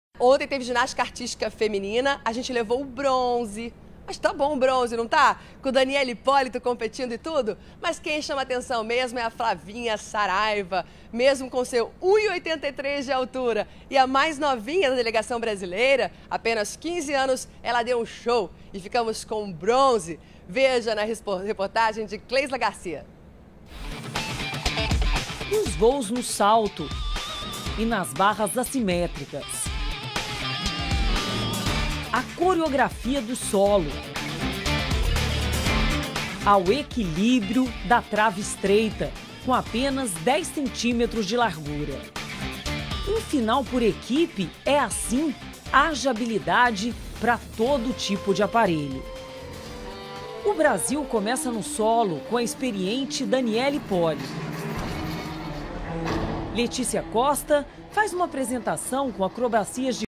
[HD] Jovem Flávia Saraiva faz Brasil levar medalha de bronze na Ginástica - Pan de Toronto 2015
Jogos Pan-Americanos de Toronto 2015
12/07/2015
Ginástica Artística
Na final por equipes da ginástica artística feminina, uma pequena gigante brasileira sobressaiu e conquistou o público do Pan de Toronto. Com 15 anos e apenas 1,33 m de altura, Flávia Saraiva arrancou aplausos com suas apresentações no solo e na barra. O Brasil terminou em terceiro lugar e ficou com a medalha de bronze.